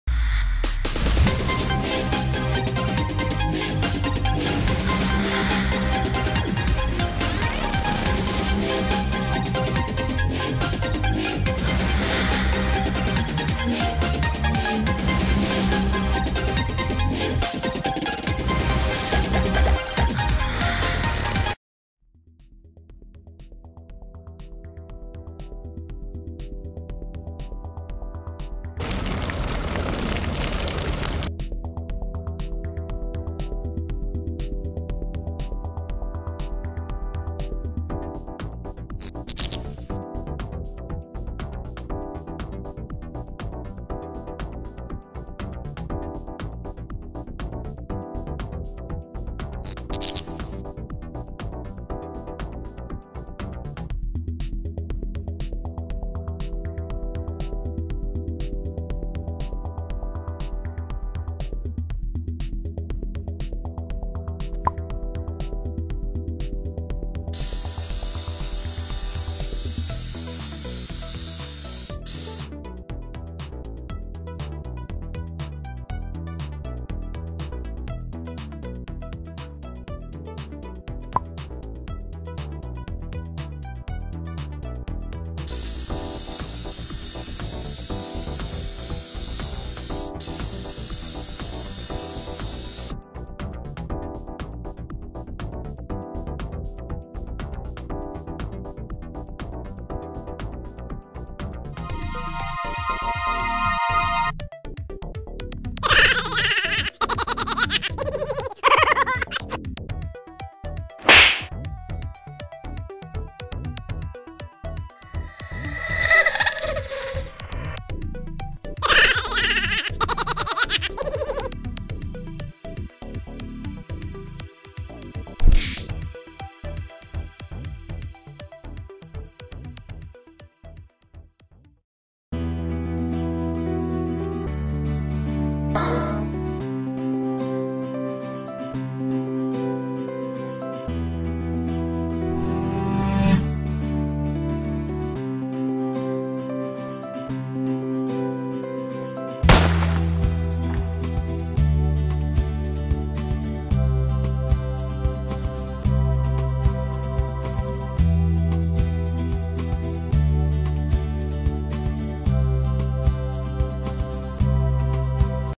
Fantastic Twins Short Movie - Part Two
This is my second short movie dedicated to my kids, Omar and Aly. I thought of that name as they are really fantastic kids. These fantastic two and short movie stars are my adobe after effects trial by using short movie ideas
CREDITS
Intro: RavenProDesign